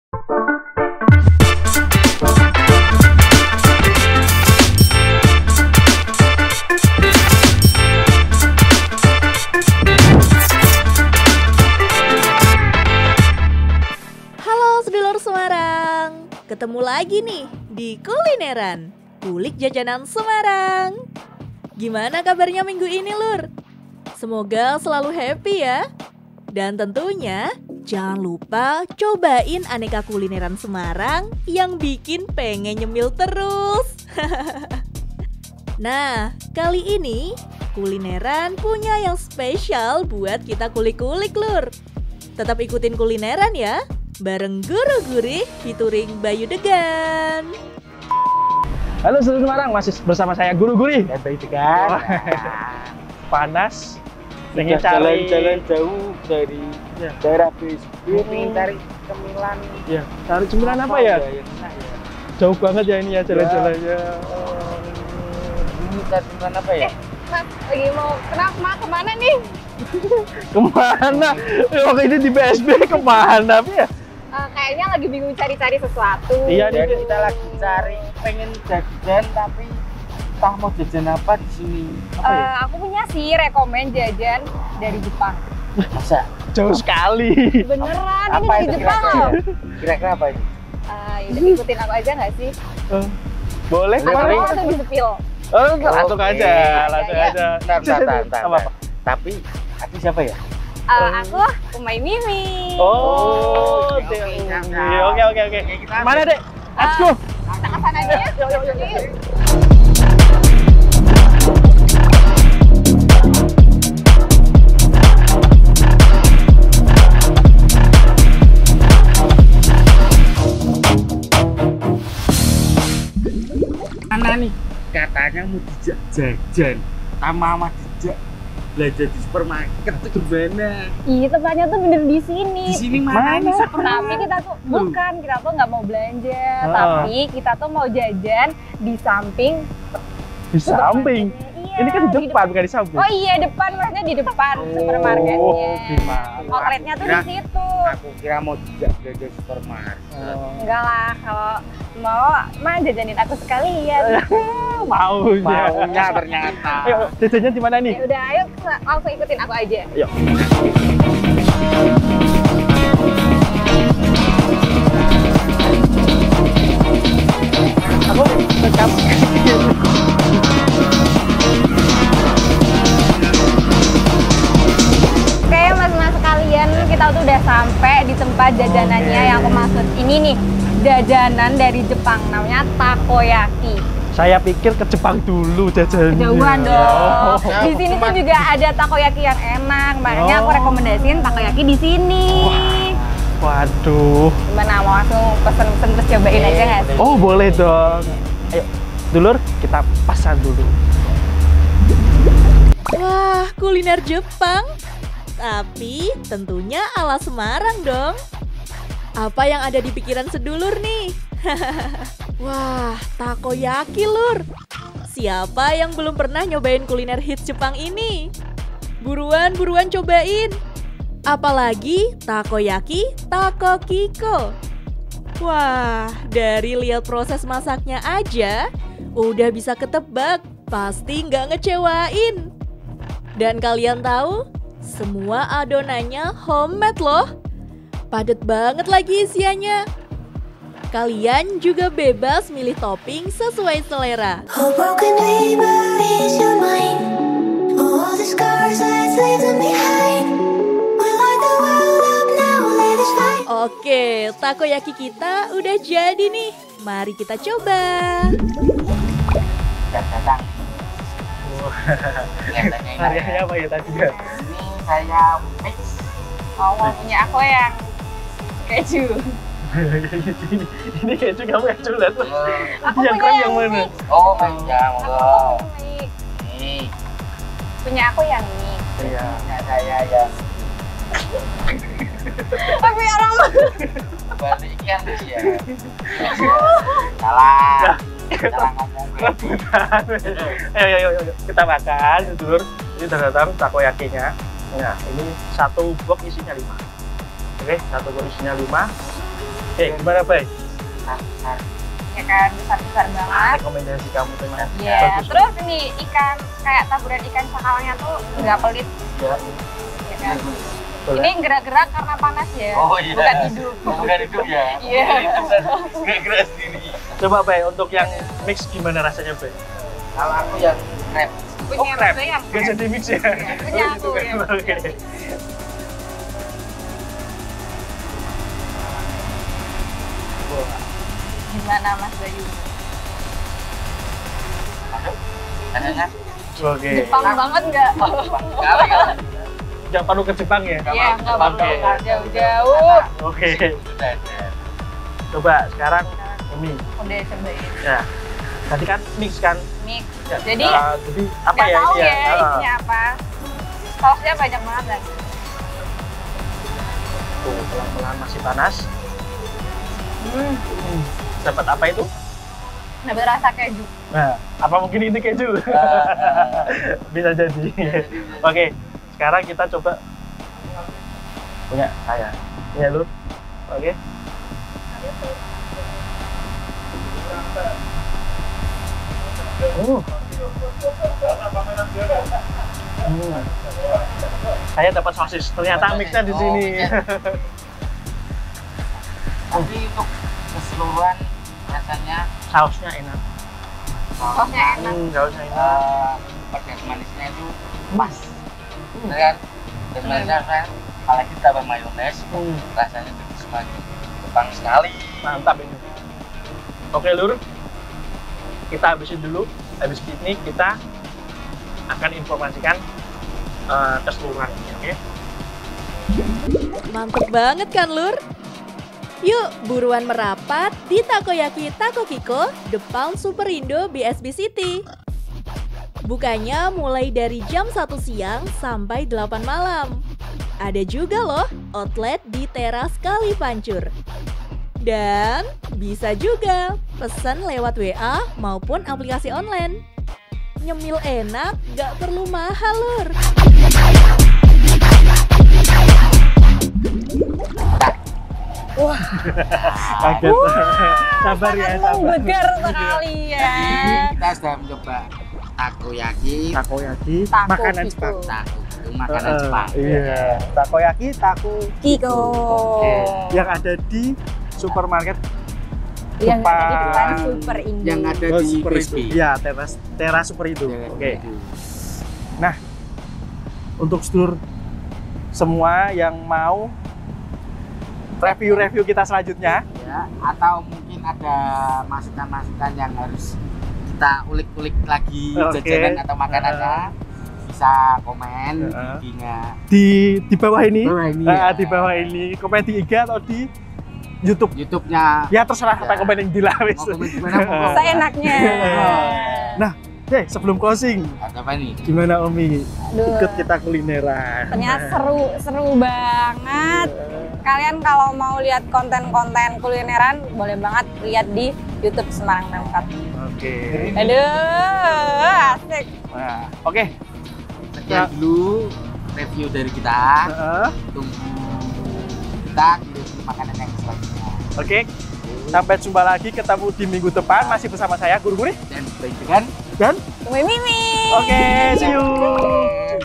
KULINERAN - TAKOYAKI TAKOKIKO
Halo, Lur!
Ada yang bulet, lembut, dan gurih, tapi bukan bakso nih!

Yap, ini adalah jajanan ala Jepang, Takoyaki!

Kulineran kali ini, mau ngajak kalian buat Takoyaki yang enak di daerah Ngaliyan, Lur! Tepatnya di depan Superindo BSB City Semarang.

Takoyaki disini uenak, Lur! Lembut dan gurih, apalagi toppingnya juga bebas pilih sendiri, wis pokoke wajib coba sendiri deh, Lur!

Nah buat yang penasaran saksikan Kulineran episode "Takoyaki Takokiko" yang tayang Sabtu, 20 Mei 2023 di YouTube Channel Semarang Pemkot.